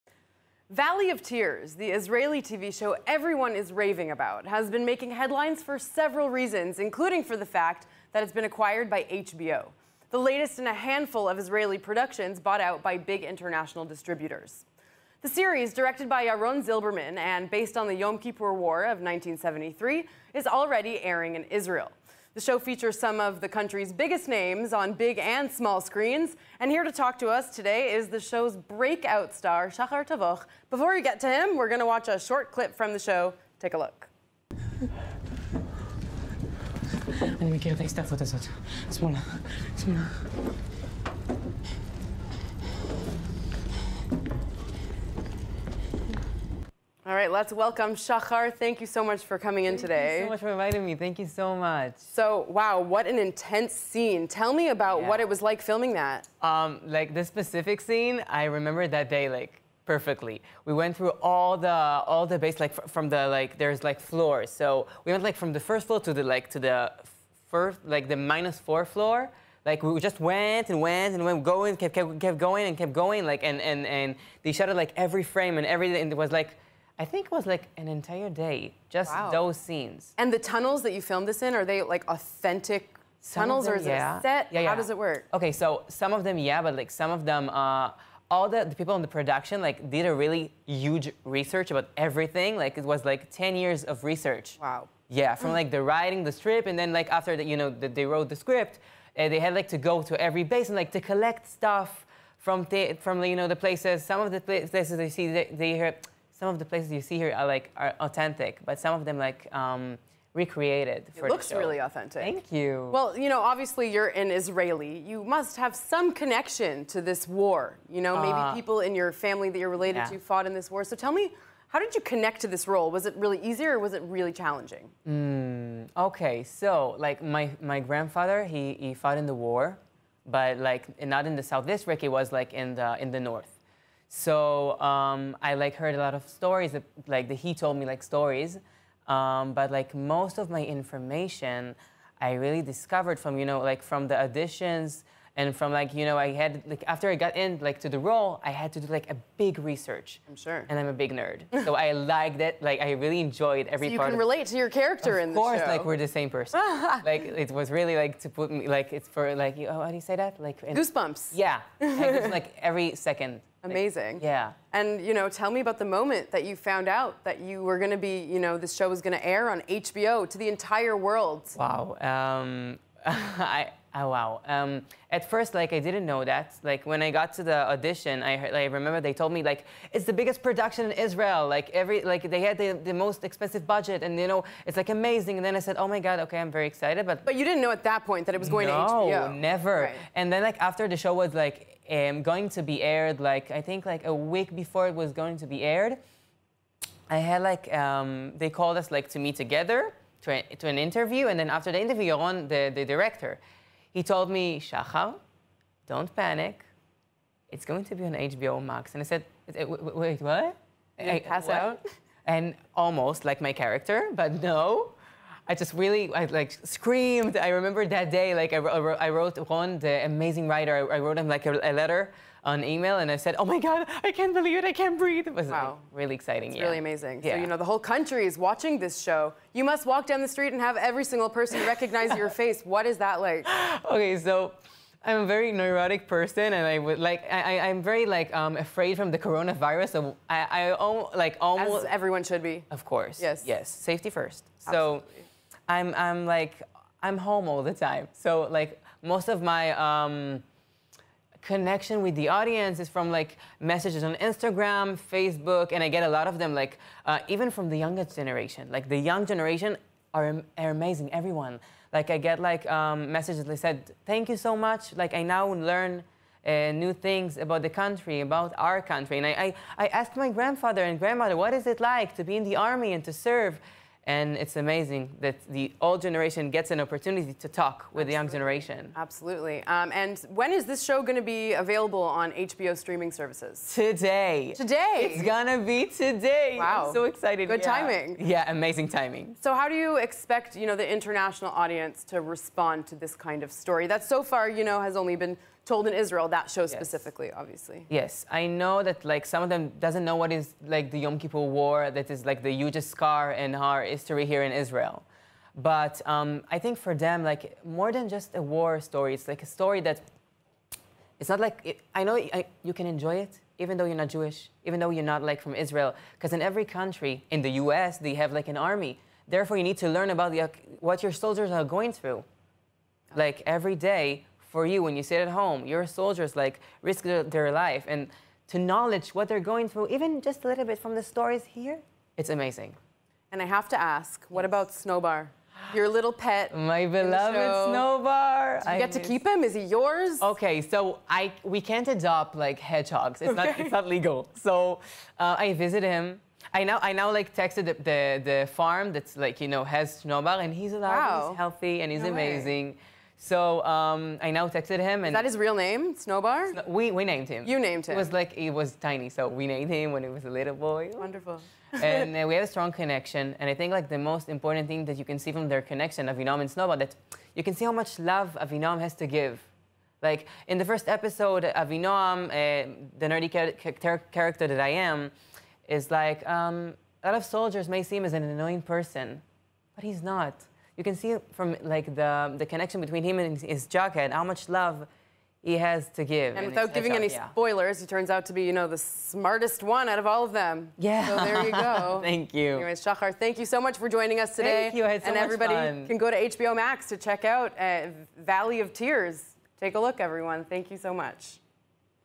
‘Valley of Tears’ - Shahar Tavoch
Israeli TV series on Yom Kippur war on the way to HBO max
Shahar Tavoch | Actor, HBO’s ‘Valley of Tears’